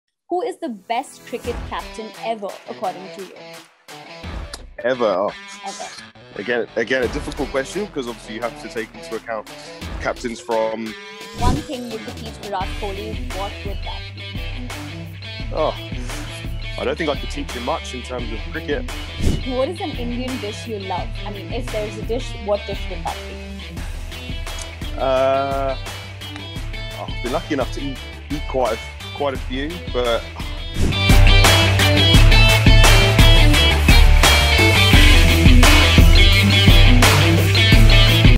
Freehit Coming Soon With England Pacer Tymal Mills
🔴 Best captain ever?
🔵 One thing he could teach Virat Kohli?
🟠 Favourite Indian dish?